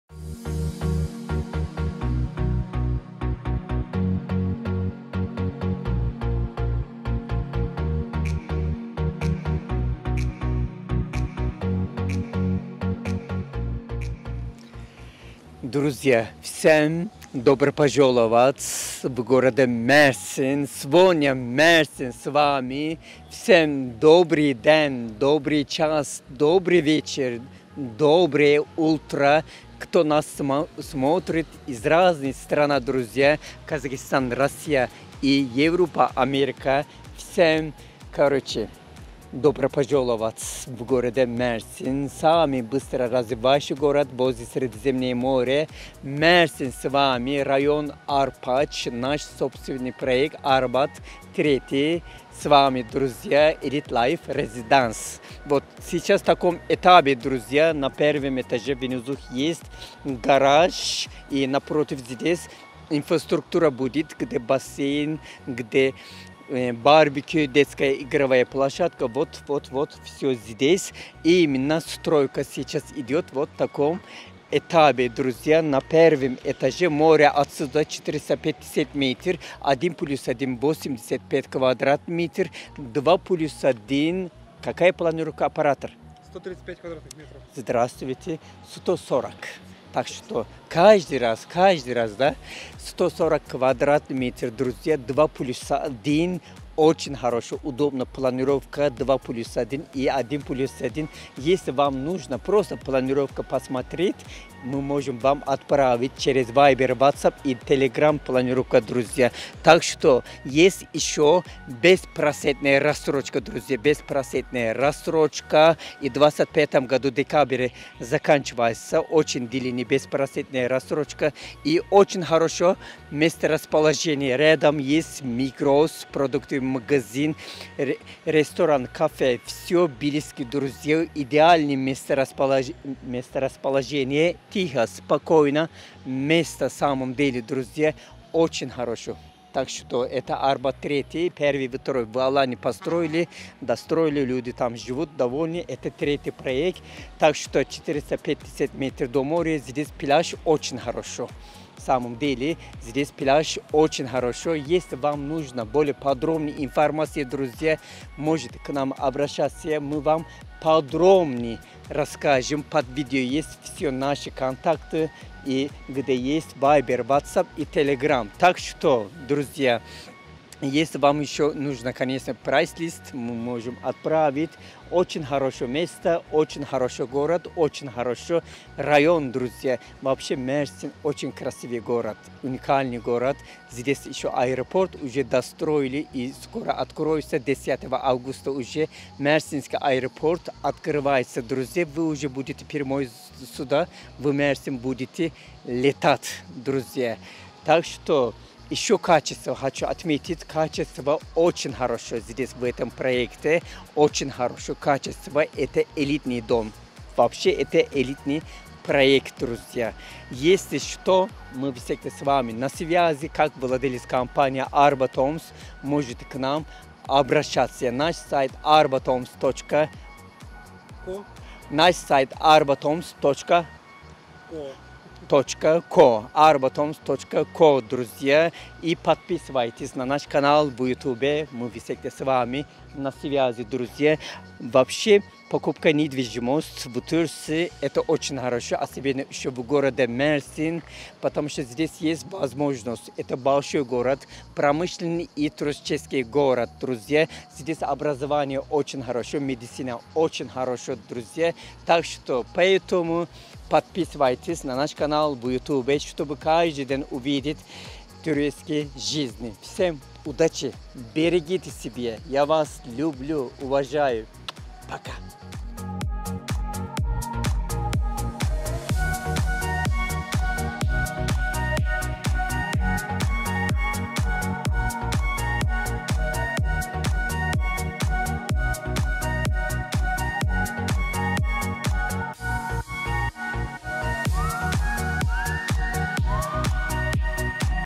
Современные Квартиры в Мерсине: Проект Arbat 3 от ArbatHomes
00:00Вступление
00:40Район Арпачбахшиш
00:52Наш проект Арбат 3
01:06Инфраструктура комплекса
01:15Расстояние до моря
01:26Планировки квартир
01:50Беспроцентная рассрочка
02:05Об инфраструктуре района
02:22Пляжи Арпачбахшиш
02:48Наши контакты
03:00Про Мерсин
04:15Заключение

Друзья, добро пожаловать в город Мерсин! С вами компания ArbatHomes и Асым Баштуг. Всем, кто нас смотрит из Америки, Казахстана, Украины, России и Европы – привет!

Мерсин – это самый быстро развивающийся город в Турции. Здесь расположен один из крупнейших портов на Средиземном море, который играет важную роль в международной торговле. В 2024 году откроется новый аэропорт Чукурова, что значительно улучшит транспортную доступность города и привлечет еще больше инвесторов и туристов.

Сегодня мы вам представляем наш проект Arbat 3, который находится в районе Арпачбахшиш. Этот проект имеет очень развитую инфраструктуру:

Аквапарк
Барбекю
Баскетбольная площадка
Беседка
Генератор
Детский бассейн
Джакузи
Закрытая парковка
Зона отдыха
Лифт
Парковка
Сауна
Фитнес
Проект находится всего в 450 метрах от моря. Планировка квартир:

1+1, площадь 85 кв.м.
2+1, площадь 140 кв.м.
Кроме того, есть беспроцентная рассрочка. Начало строительства запланировано на декабрь 2023 года, а окончание – на декабрь 2025 года. Высота здания составит 13 этажей.

Район Арпачбахшиш в Мерсине продолжает набирать популярность среди иностранных инвесторов. Городские власти активно развивают район, проводя озеленение набережной и создавая красивый парк. На набережной можно прогуляться и покататься на велосипедах и роликах, так как это исключительно пешеходная зона, где запрещено движение автомобилей.

Пляжи в Арпачбахшиш, Томюке и Чешмели – это длинная пляжная полоса, полностью оборудованная лежаками и интересными соломенными зонтиками. Здесь есть все для вашего удобства. В этих районах также расположено много ресторанов рыбной кухни, которые находятся прямо на набережной.

Если у вас есть вопросы или вы хотите узнать больше о проекте Arbat 3, вы можете обращаться к нам по номеру, и мы с радостью подробно расскажем и покажем все, что вас интересует.

Не пропустите новые видео! Не забывайте делиться этим видео с теми, кому оно может быть полезным. До скорой встречи, друзья! С вами был канал 'ArbatHomes - Недвижимость в Турции'.